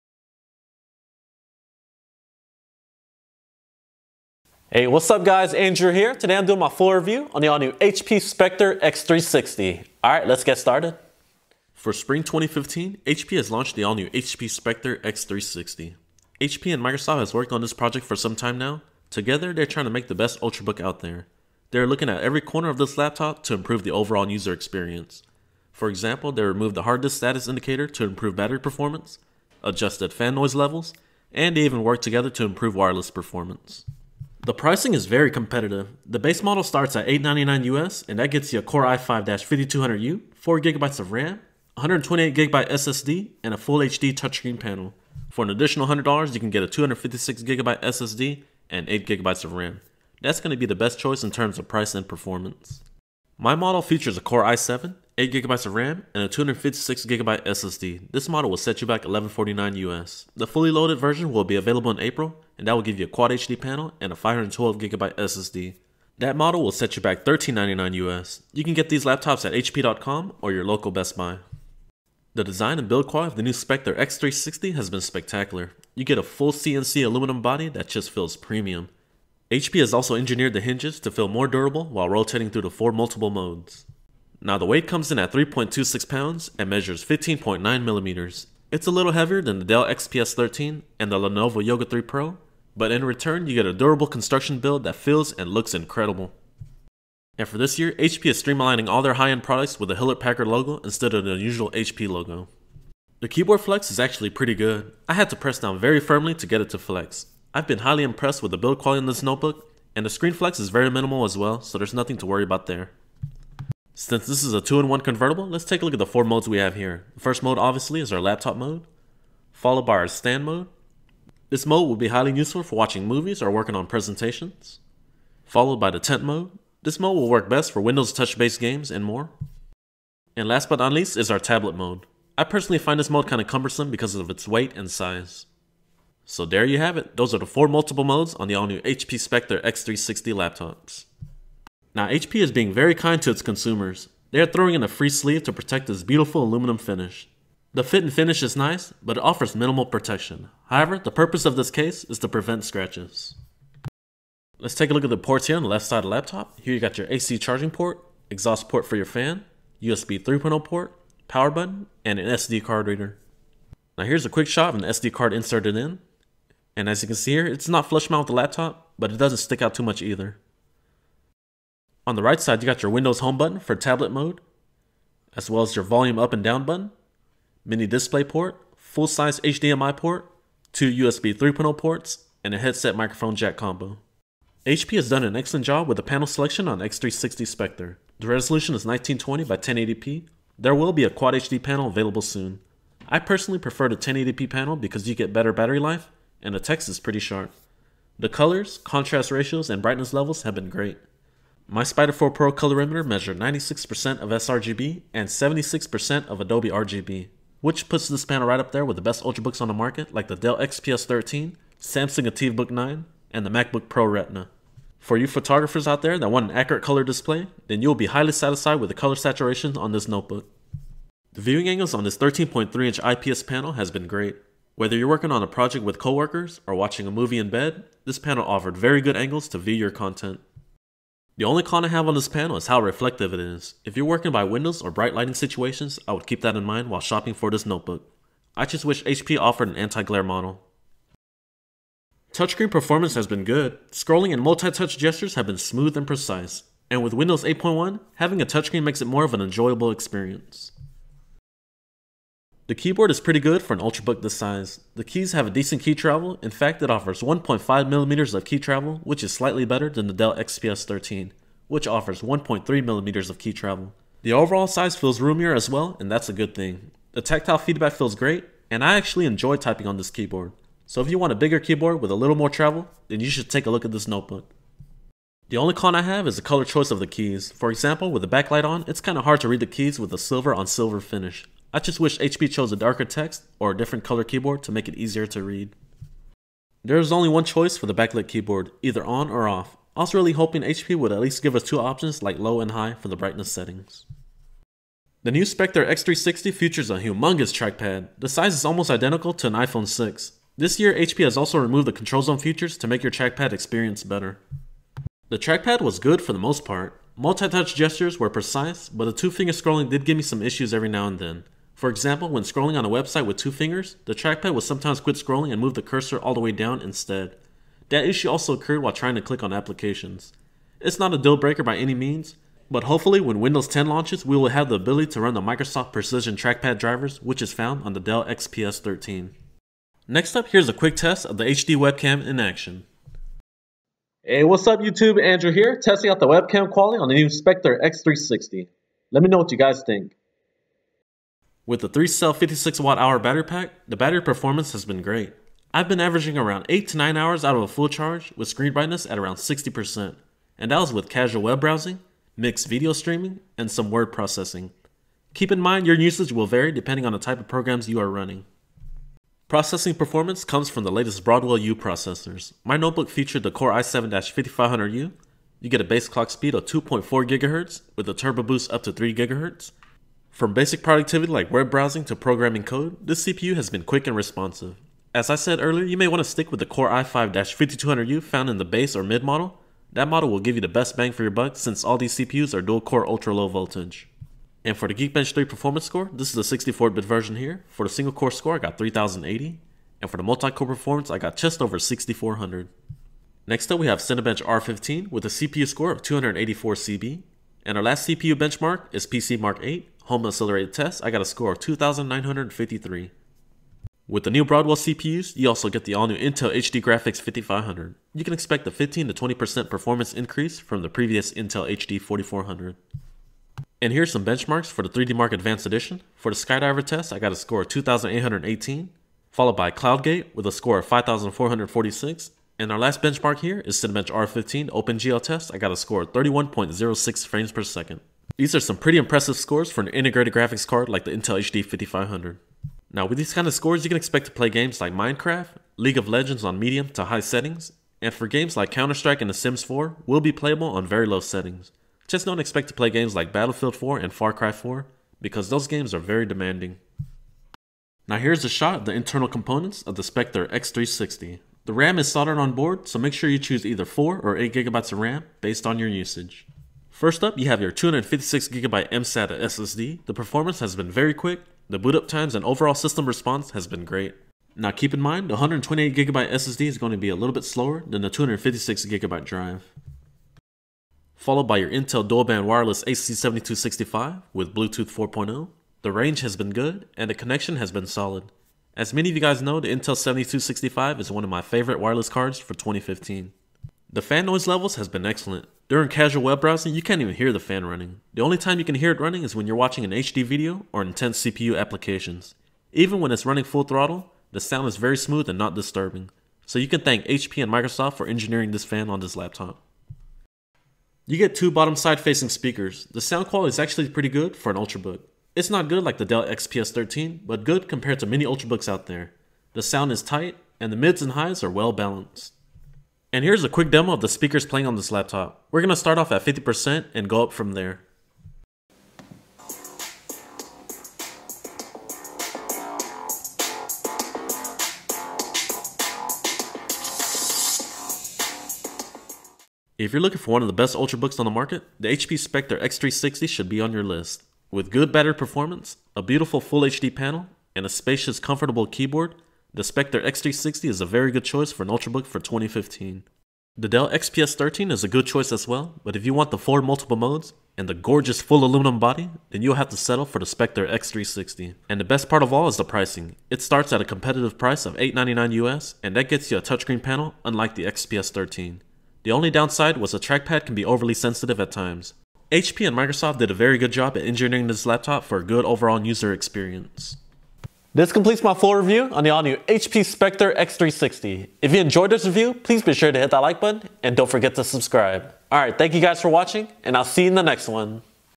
HP Spectre x360 Review 2015
What's up YouTube, here is my full review on the all new HP Spectre x360 ultrabook. HP and Microsoft has been working together on trying to deliver the best ultrabook out there. Did the deliver? Lets find out...

Specs:

13.3" FHD Touchscreen panel
Intel Core i7-5500U @ 2.4GHz with turbo boost up to 3.0GHz
8GB of RAM
256GB mSATA SSD
Intel Dual Band Wireless AC 7265 + Bluetooth 4.0
3 USB 3.0 ports
HDMI
Mini Display port
HD Webcam
56 WHr Battery
$1149

1 Year Warranty